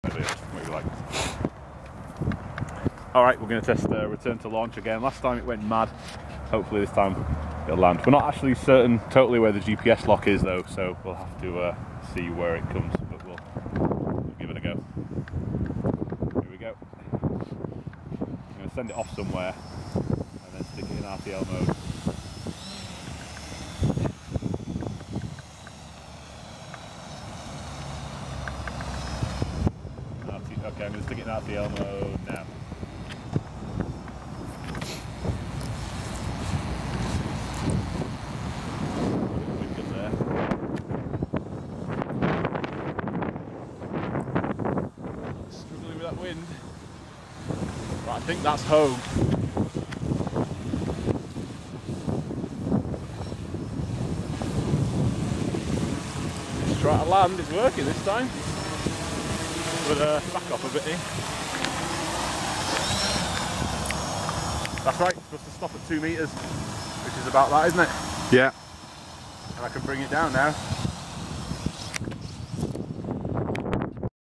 Quadcopter successful "return to launch" test
A test of the "return to launch" feature, which should bring the aircraft back home. It should go up above tree level, move to the gps coordinates of the launch site, hover for 5 seconds in place, and then drop to 2m from the ground and hover there.

Last time I tried this, it flew off in the wrong direction and hit a tree. This time there is more space to see what it tries to do.

It worked pretty much perfectly this time (though on a later run, it went off and tried to land way off mark - so I suspect the gps lock just wasn't accurate enough).